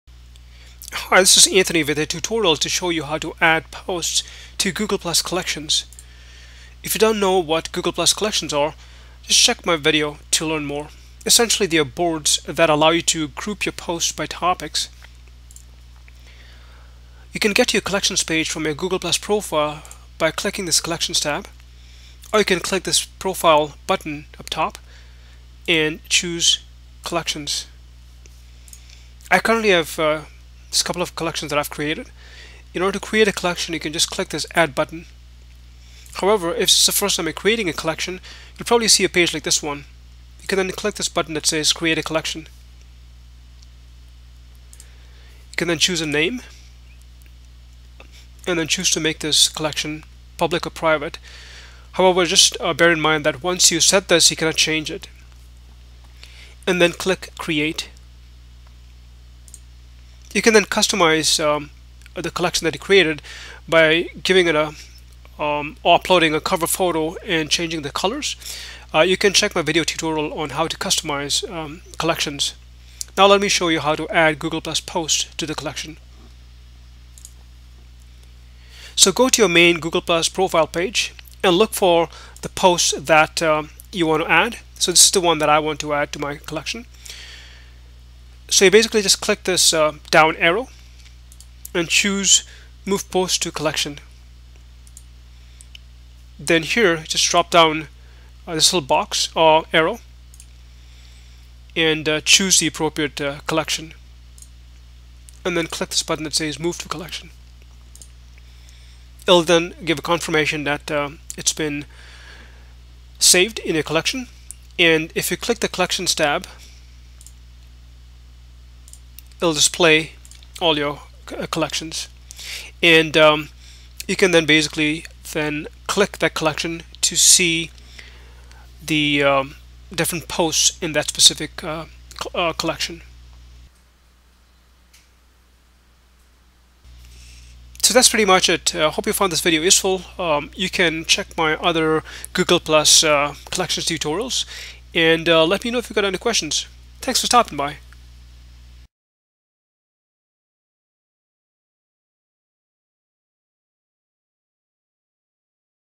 How to Add Posts to Google+ Collections
In this tutorial, you will learn how to add posts to Google+ Collections. By grouping your posts using the Collections features, you can organize your Google+ posts and help your readers find posts more easily. Google introduced Collections in May 2015.

CREDITS:

Google and the Google logo are registered trademarks of Google Inc., used with permission.